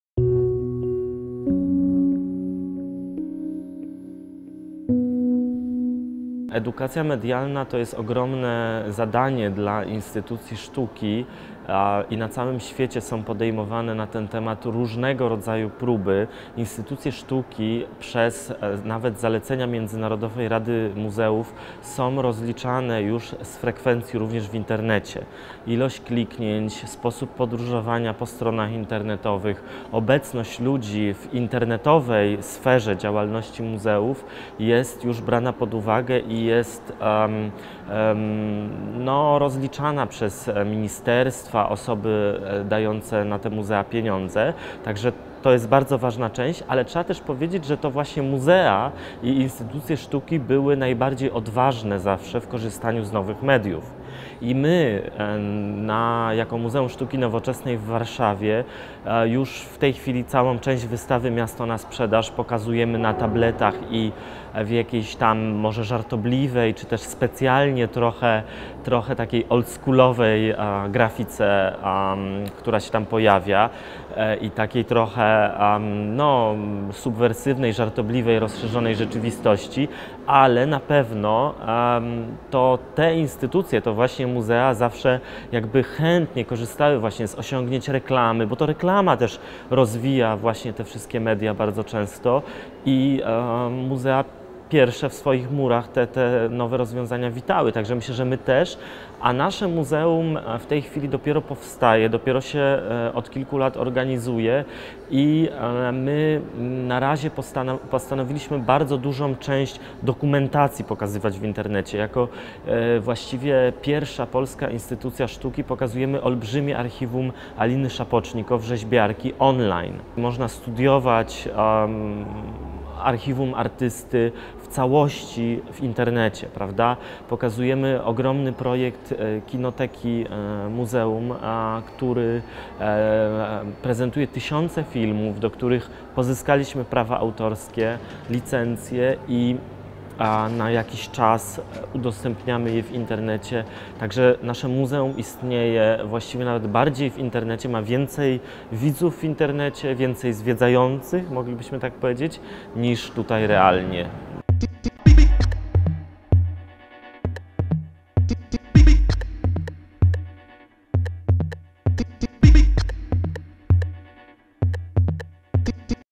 O edukacji medialnej mówi Tomasz Fudala kurator MSN
O edukacji medialnej mówi Tomasz Fudala - kurator wystawy "Miasto na sprzedaż" w warszawskim Muzeum Sztuki Nowoczesnej.

Więcej na Civilia.pl